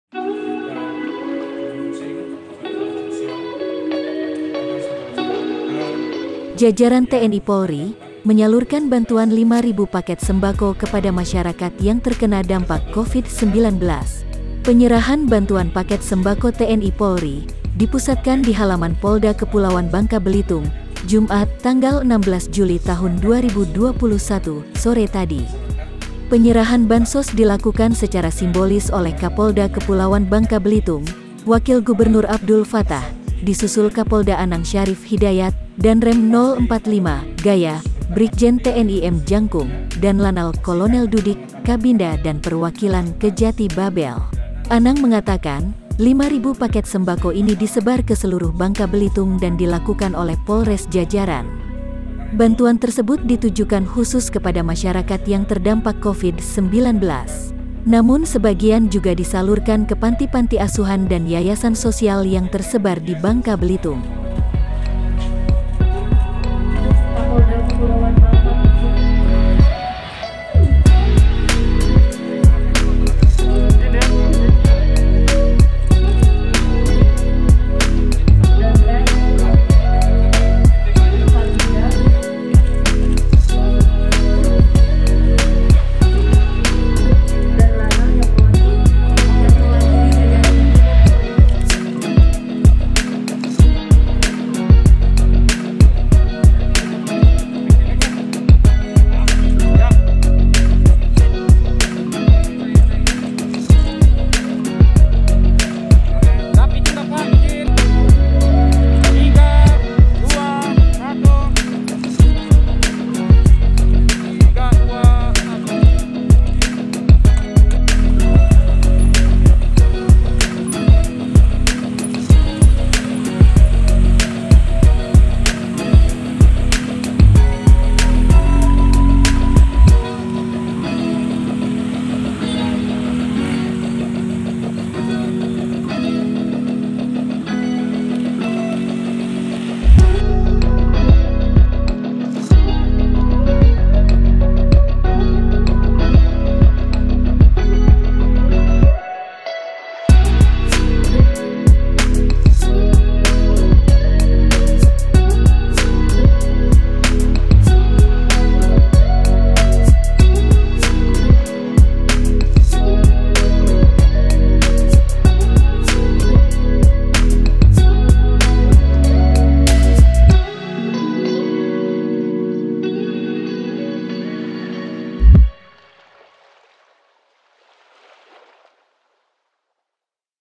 Polda Babel Sebar 5.000 Paket Sembako ke Warga Terdampak Covid 19
BANGKAPOS.COM, BANGKA - Jajaran TNI Polri, menyalurkan bantuan 5.000 paket sembako kepada masyarakat yang terkena dampak covid 19.

Penyerahan bantuan paket sembako TNI Polri, di pusatkan di halaman Polda Kepulauan Bangka Belitung, Jumat (16/7/2021) sore tadi.

Penyerahan bansos dilakukan secara simbolis oleh Kapolda Kepulauan Bangka Belitung, Wakil Gubernur Abdul Fattah, disusul Kapolda Anang Syarif Hidayat, Danrem 045/Gaya, Brigjen TNI M Jangkung, Danlanal Kolonel (L)  Dudik,  Kabinda dan Perwakilan Kejati Babel.

Anang mengatakan, 5.000 paket sembako ini disebar ke seluruh Bangka Belitung dan dilakukan oleh Polres jajaran.

Bantuan tersebut ditujukan khusus kepada masyarakat yang terdampak covid 19. Namun sebagian juga disalurkan ke panti panti asuhan dan yayasan sosial yang tersebar di Bangka Belitung.

" Pada prinsipnya semua Polres juga melakukan hal yang sama.
totalitas untuk seluruh Bangka Belitung dengan 7 Polres itu ada sebanyak 5000 paket dan ini kita sebarkan kepada masyarakat khususnya ditujukan kepada masyarakat yang terdampak covid-19," kata Anang, Jumat (16/7/2021) sore tadi